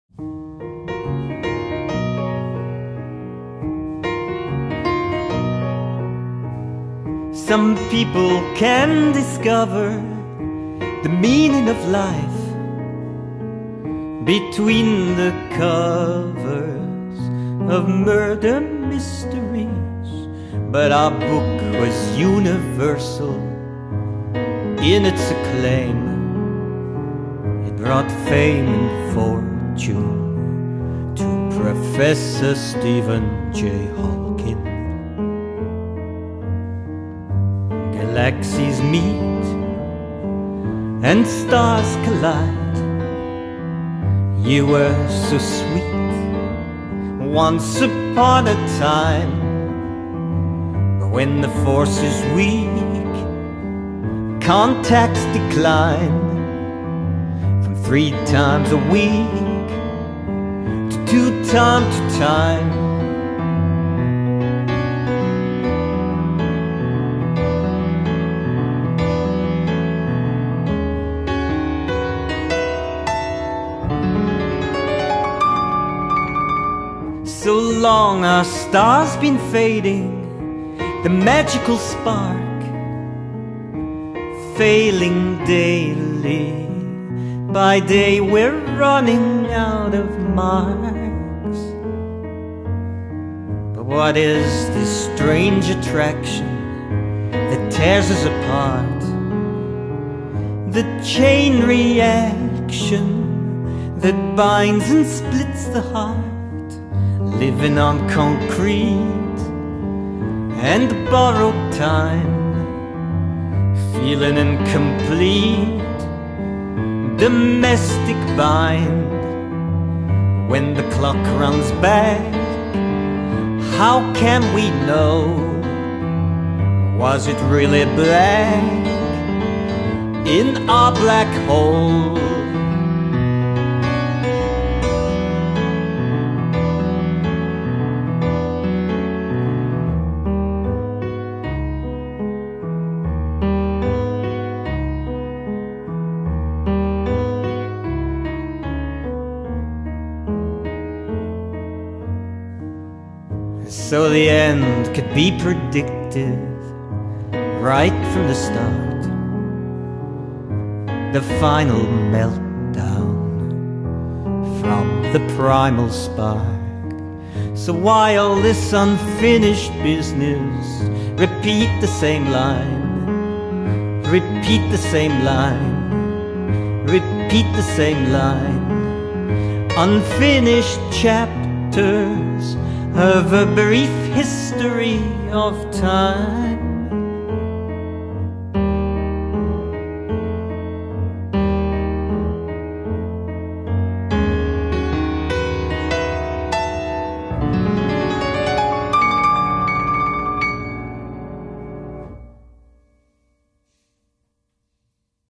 A Brief History of Time
Mr Diagonal's special homage to prof Stephen J Hawking RIP.
Video by Ken Nishikawa.